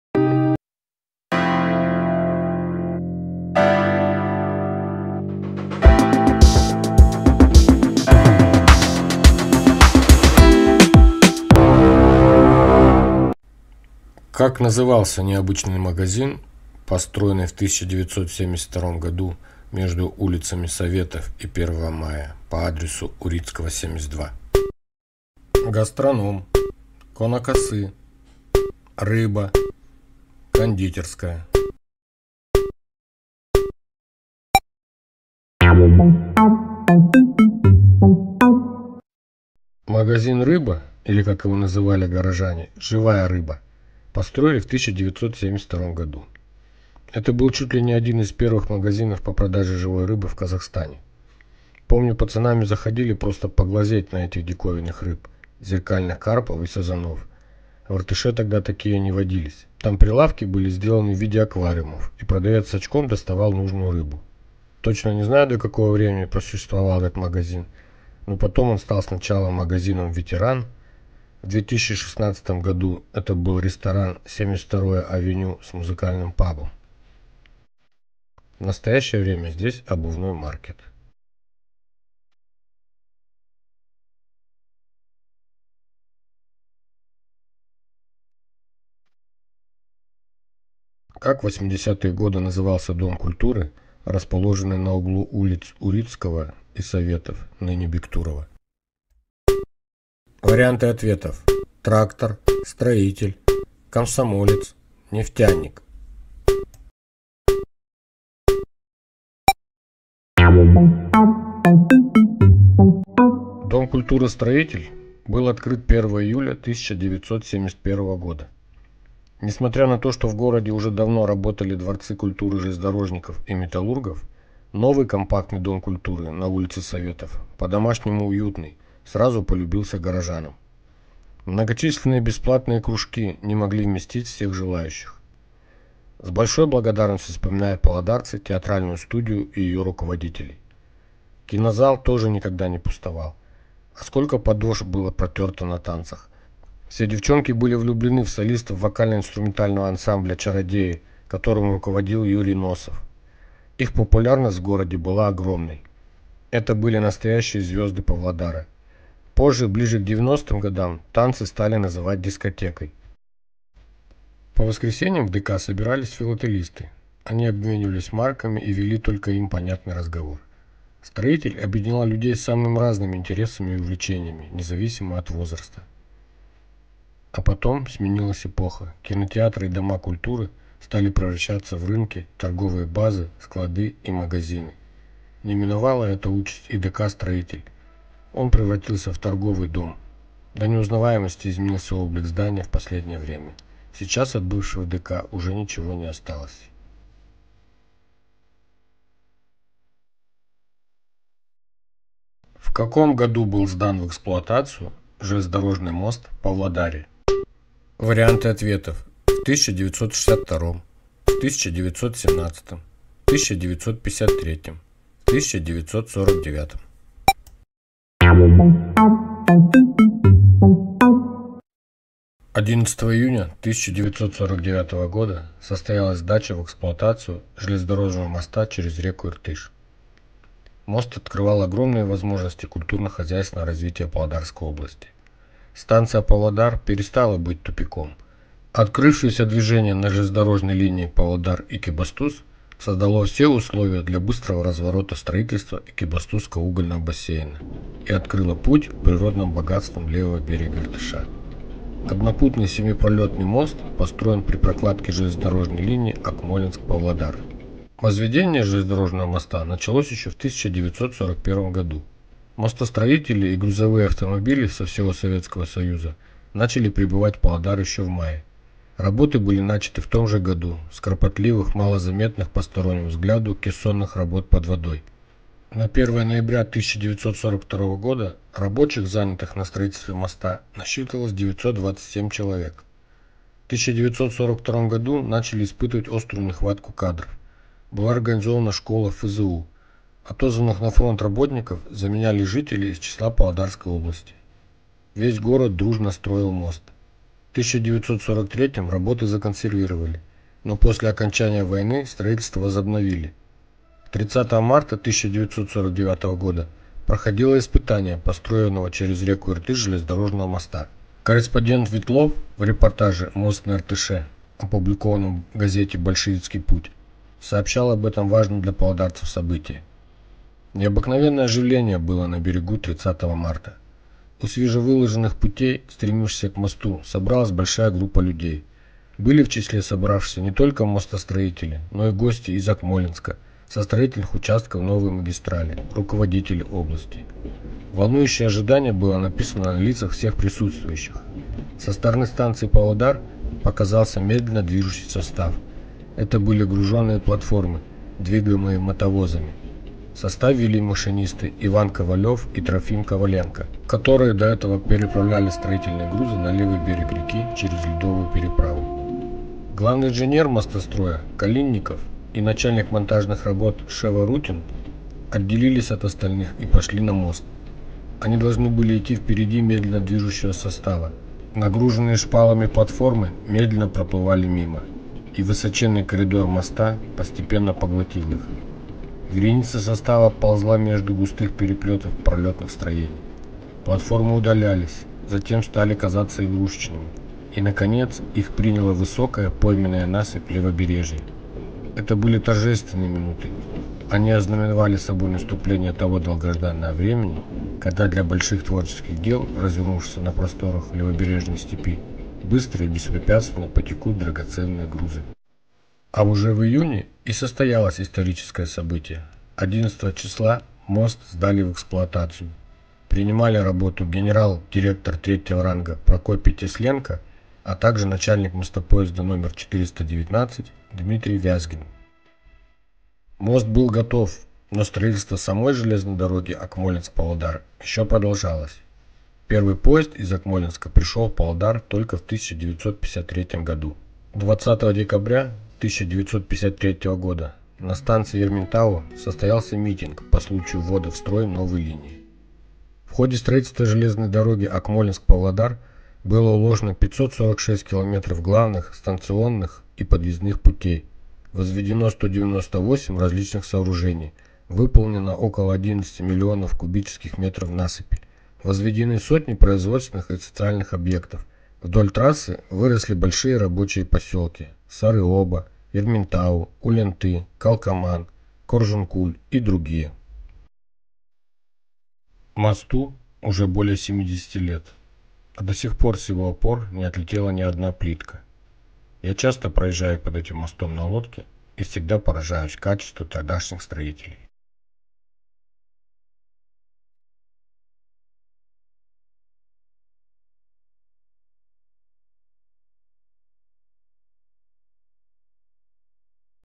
Знаете ли вы Павлодар? Интересные истории и факты.Второй выпуск.
Небольшая викторина на знание Павлодара. Павлодарцы- проверьте свои знания о городе Павлодар.
ЮMoney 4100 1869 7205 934
КаспийГолд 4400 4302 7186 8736
Буду продолжать делать для Вас качественнный контент.